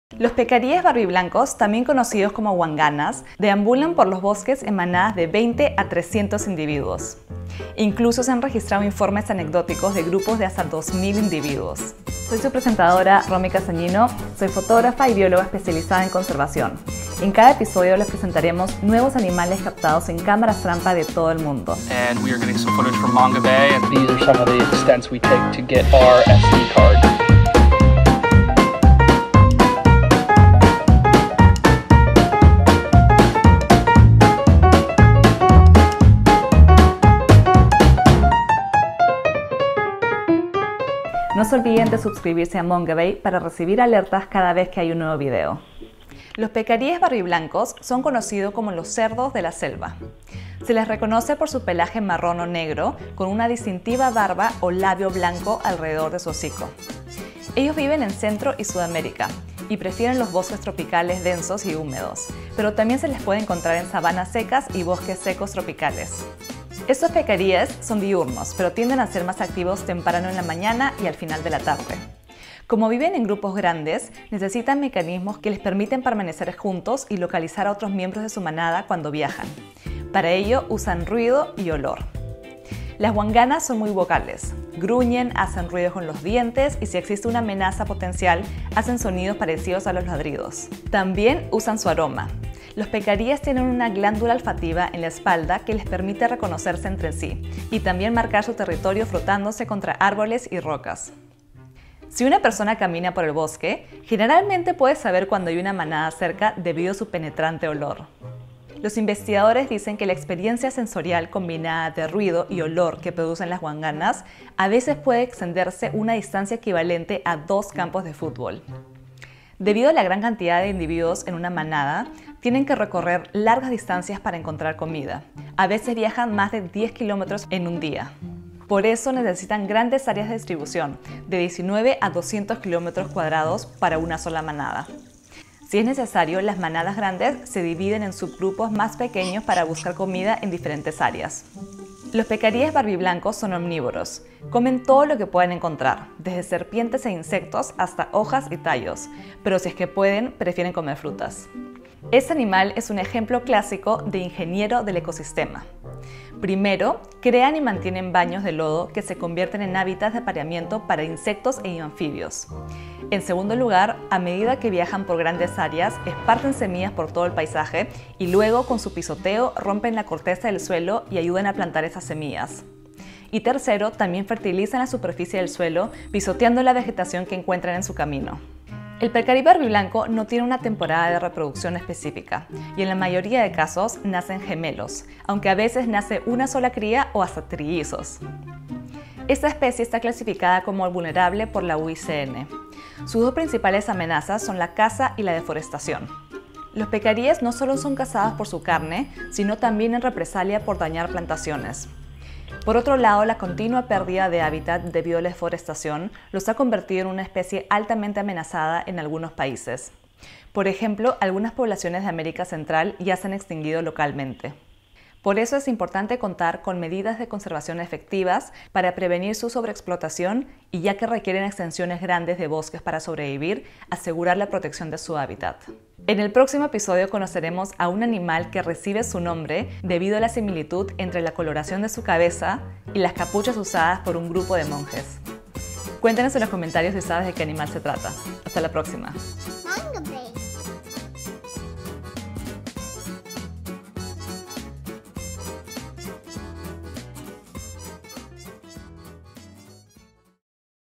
Conozcamos al pecarí barbiblanco en Candid Animal Cam | Episodio 39
¿Sabías que los pecaríes barbiblancos deambulan por los bosques en manadas de 20 a 300 individuos? En un nuevo episodio de CandidAnimalCam les contamos todo sobre esta especie que vive en Centro y Sur América. Estos animales también son llamados "cerdos de la selva" y se les reconoce por su pelaje marrón o negro con una distintiva barba o labio blanco alrededor del hocico. ¿Qué otra especie quieres conocer? Participa en los comentarios.